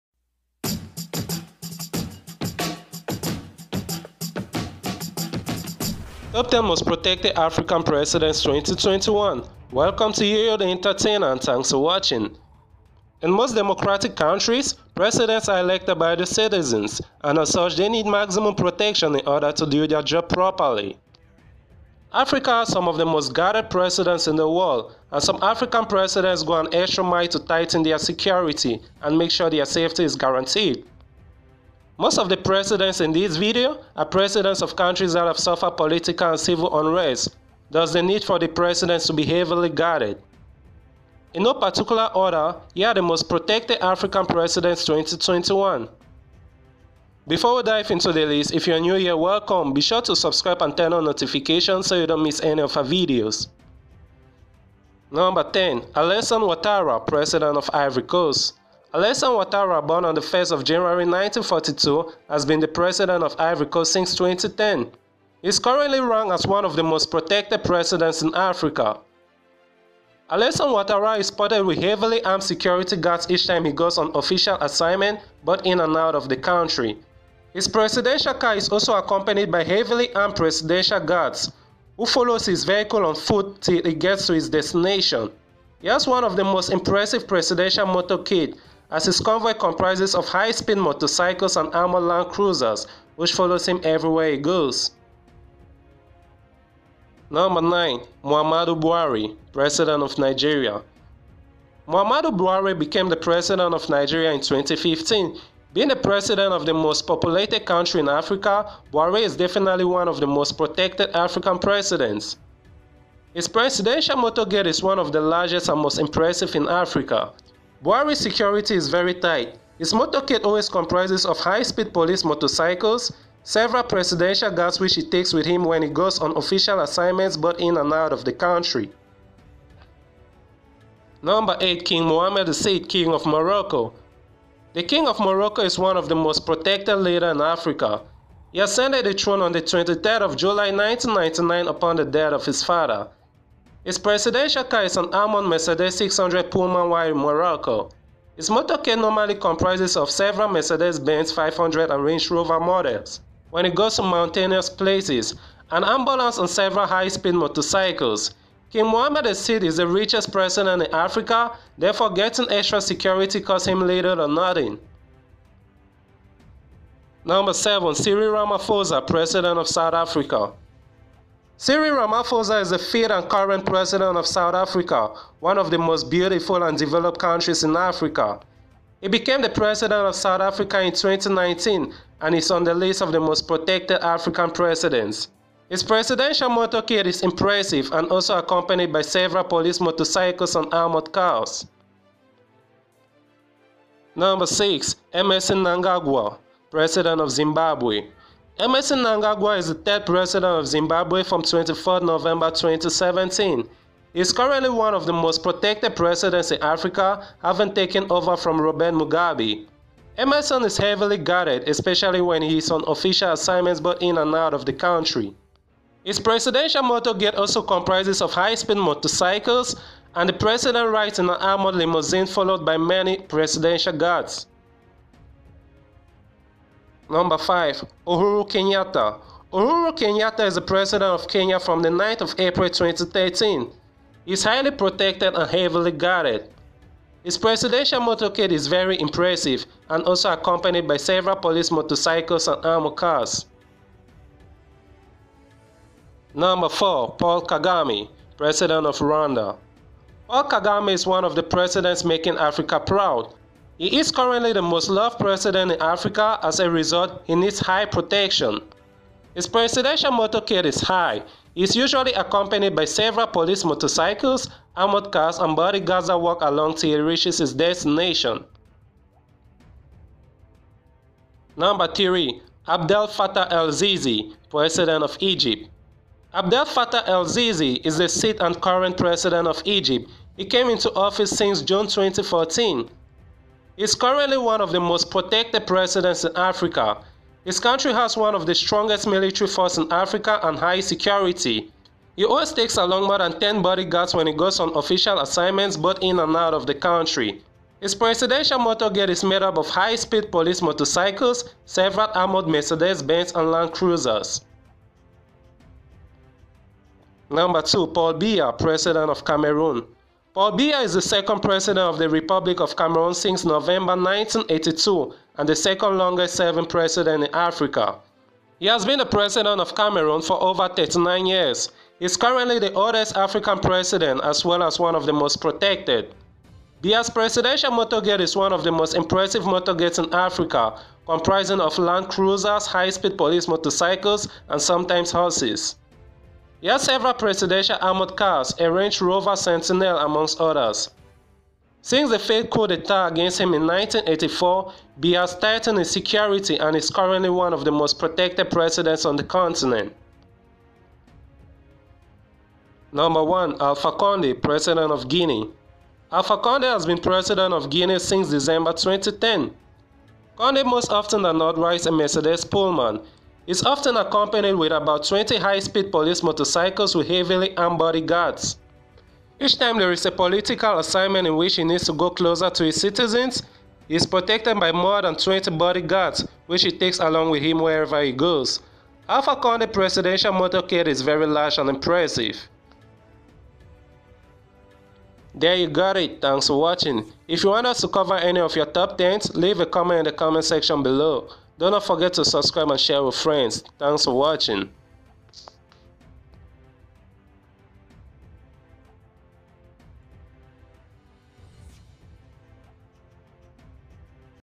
Top 10 Most Protected Presidents In Africa 2022 (They Got The Highest Security In The World)
Top 10 Most Protected President In Africa 2022

In most democratic countries presidents are elected by the citizens, And as such, they need maximum protection in order to do there job properly. Africa has some of the most guarded presidents in the world, and some african presidents go an extra mile to tighten their security and make sure their safety is guaranteed. Most of the presidents in this video are presidents of countries that suffer political and civil unrest thus the need for the presidents to be heavily guarded.

10. Allasane Ouatara – President of Ivory Coast
Allasane Ouatara born on the 1st of January 1942 has been the president of Ivory coast since 2010. He currently ranks as one of the most protected presidents in Africa. Allasane ouatara is also spotted with heavily armed security guards each time he goes on official assignment both in and out of the country. His presidential car is also accompanied by heavily armed presidential  guards who follows his vehicle on foot until it gets out of the reach of the public. He has one of the most impressive Presidential. Motorcade as his convoy comprises of High speed motorcycles and Armored Land cruisers which follows him wherever he goes.

9. Muhammadou Buhari – President of Nigeria
Muhammadou Buhari became the president of Nigeria in 2015, Being the president of the most populous country in Africa, Buhari is definately one of the most protected presidents in Africa. His presidential Motorcade is one of the largest and most impreesive in Africa. Buhari’s security is very tight,
His Motorcade always comprises of high speed police motorcycle, several presidential guards which he takes with him when he goes on Official Assignments both in and out of the country.

8. King Mohammed VI – King Of Morroco
The king of morocco is one of the most protected leader in Africa. He ascended the throne on the 23rd of July 1999 upon the dead of his father.
His presidential car is an Amoured Mercedez 600 Pullman while in morocco. His motorcade normally comprises of a several mercedez bens 500 and Range Rover models when he goes to mountainous places, an ambulance and several high speed motocycles. King Mohommed VI is the richest president in Africa therefore getting extra security costs him little or nothing.

7. Cyril Ramaphosa - President of South Africa
Cyril Ramaphosa is the fifith and current president of South Africa, one of the most beautiful and developed countries in Africa. He became the president of south Africa in 2019 and is on the list of the most protected African presidents. His presidential Motorcade is impressive and also accompanied by several police motorcycles & armored cars.

6. Emmerson Mnangagwa – President of Zimbabwe
Emmerson Mnangagwa is the 3rd president of Zimbabwe from 24 November 2017. He is currently one of the most protected presidents in Africa haven’t taken over from Robert Mugabe. Emmerson is heavily guarded especially when he is on officials assignments both in and out of the country. His presidential motorcade also comprices of high speed police motorcycles and the president rides in an Armoured limosine followed by many presidential guards.

5. Uhuru Kenyatta
Uhuru Kenyatta is the president of Kenya from 9th April 2013, he is highly protected & heavily guarded. His presidential motorcade is very impressive and also accompanied by several police motorcycles and armored cars

4. Paul Kagame - President of Rwanda.
Paul Kagame is one of the Presidents making Africa proud. He has transformed uganda to one of the fastest developing country, most peaceful and fast growing economy in Africa. He is currently the most loved president in Africa as a result the need for high protection. His presidential motorcade is high, he is usually accompanied by several motorcycles, armored cars and body guards that walk along till he reaches his destination
3. Abdel Fattah El-Sis -- President of Egypt
Abdel Fattah el-Sisi is the sixth and current President of Egypt. He came into office since June 2014. He is currently one of the most protected presidents in Africa, his county has one of the strongest military force in Africa and high security, he always takes along more than 10 bodyguards when he is on official assignment both in and out of the country
His presidential motorcade is made up of high speed bikes, several armored mecerdez Bens and land cruisers

2. Paul Biya - President of Cameroon
Paul Biya is the 2nd president of the republic of Cameroon since November 1982, and the 2nd longest serving president in Africa. He has been president of Cameroon for over 39 years. He is currently the oldest president in Africa as well as one of the most protected. Biya's presidenti